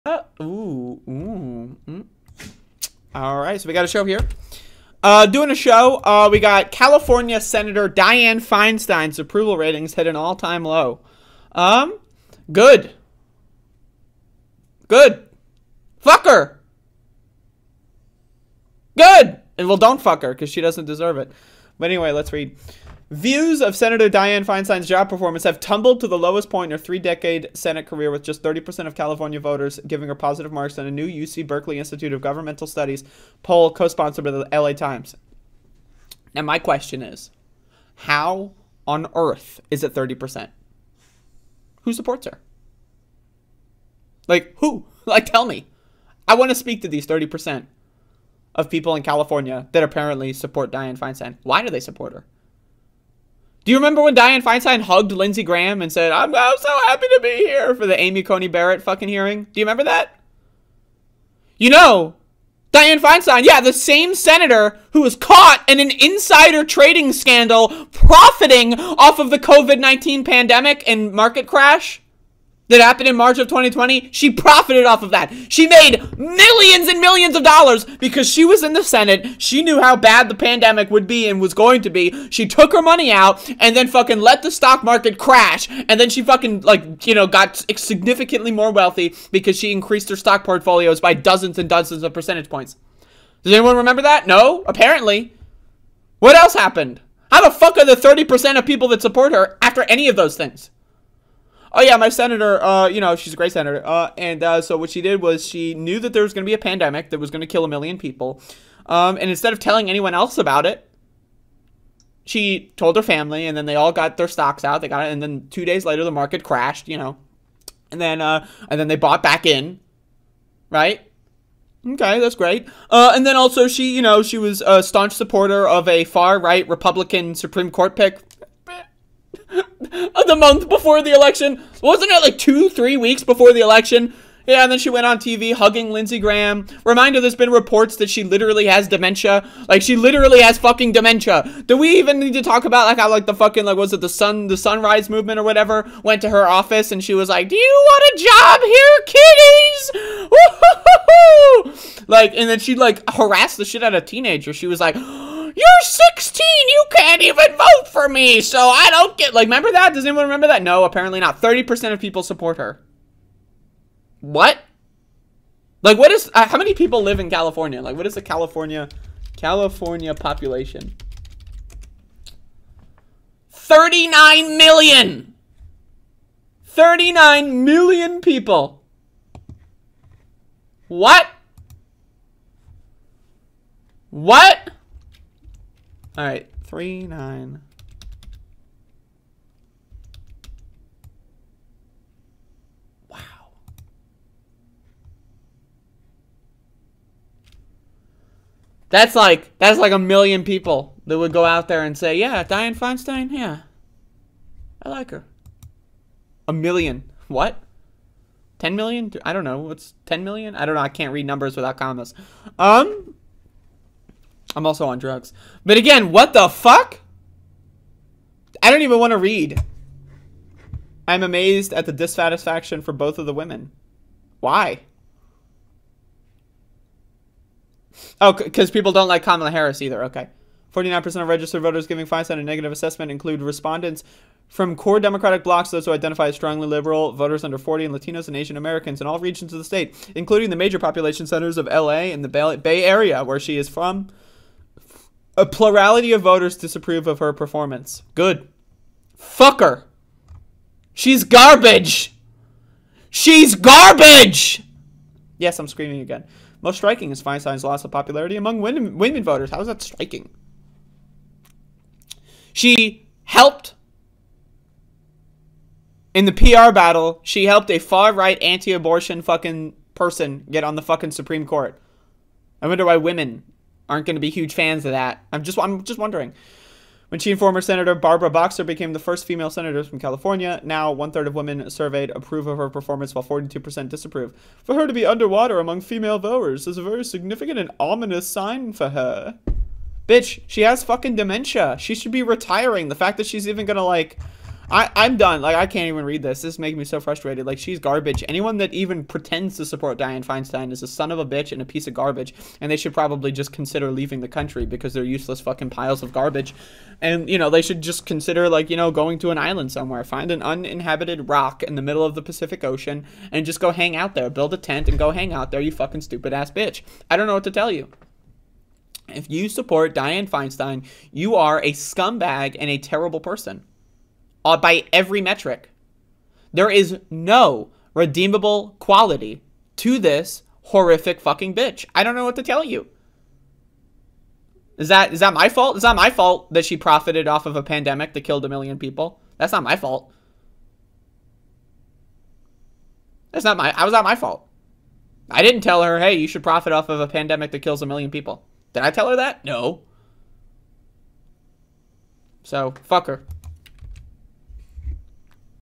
Dianne Feinstein's Approval Rating Drops To Stunning 30%
Send me money to help me eat tomorrow!